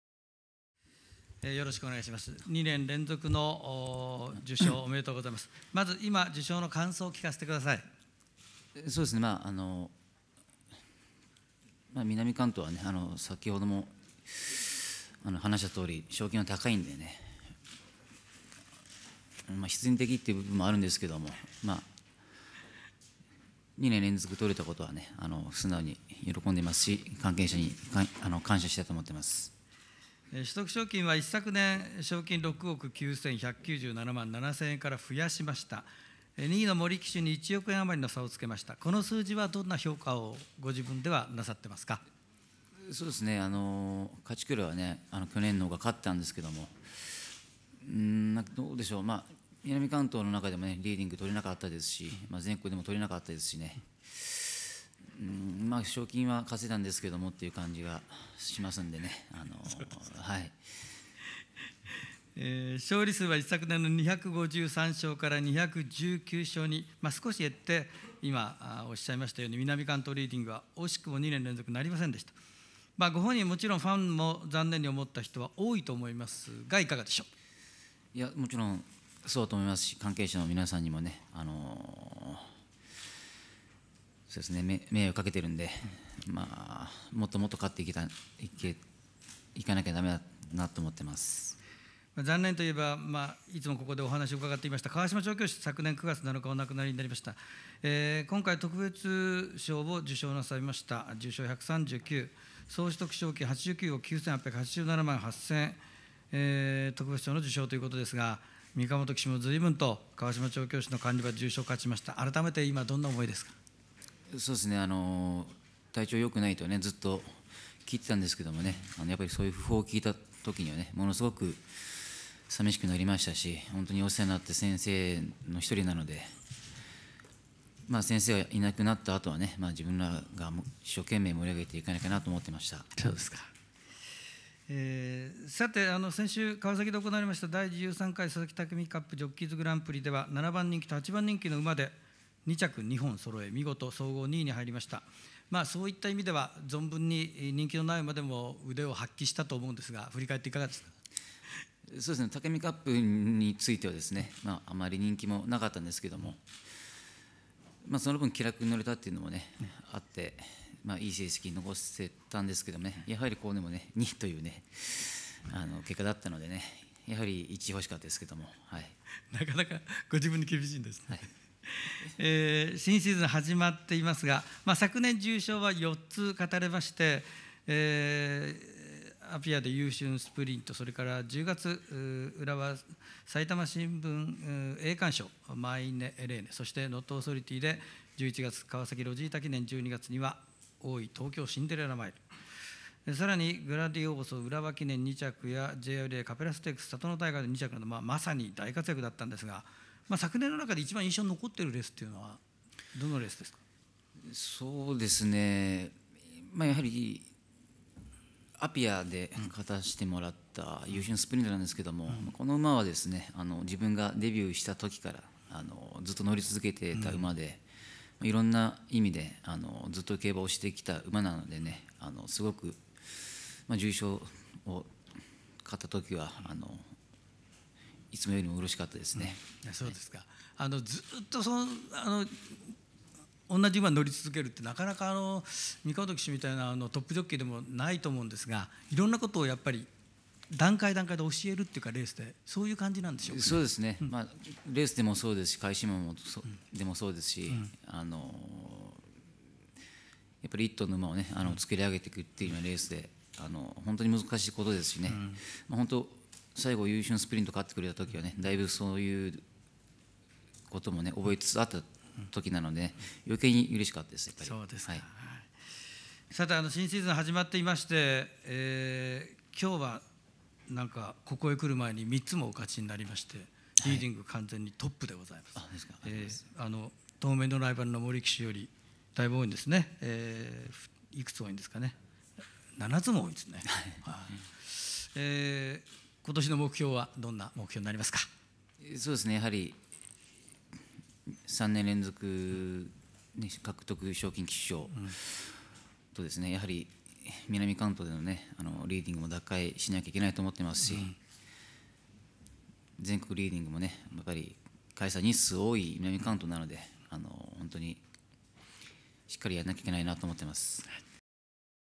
【NARグランプリ2014②】最優秀賞金収得騎手賞 御神本訓史騎手
最優秀賞金収得騎手賞には、TCK御神本訓史（みかもとのりふみ）騎手が選ばれました。（2年連続2回目）
記者会見をご紹介します。